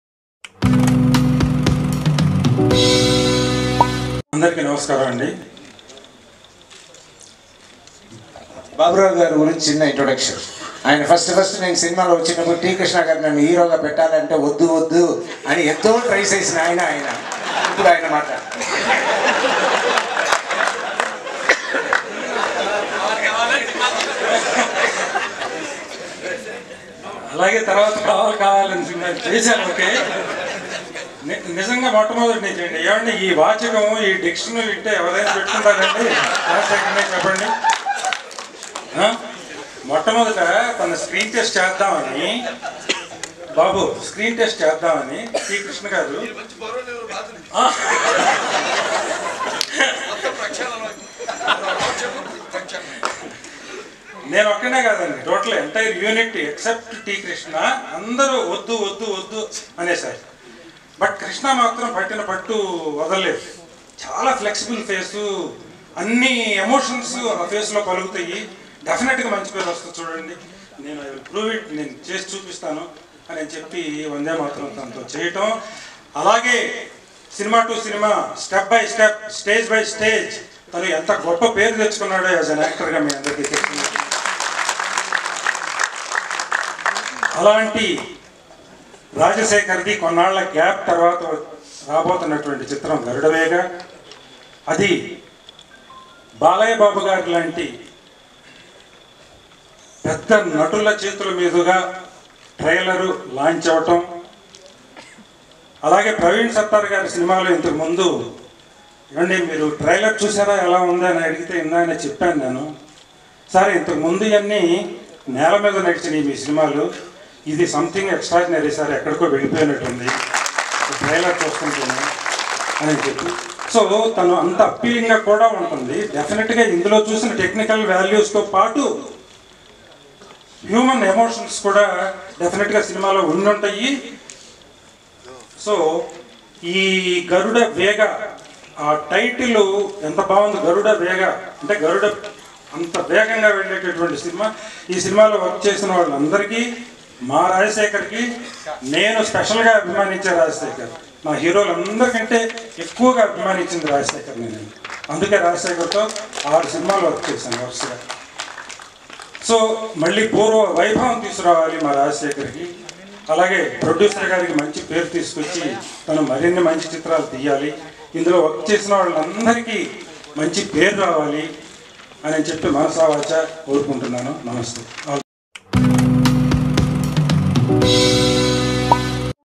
Garuda Vega Trailer Launch : Pokuri Babu Rao Speech | Filmibeat Telugu
Watch Pokuri Babu Rao Speech at Garuda Vega Trailer Launch
Filmibeat Telugu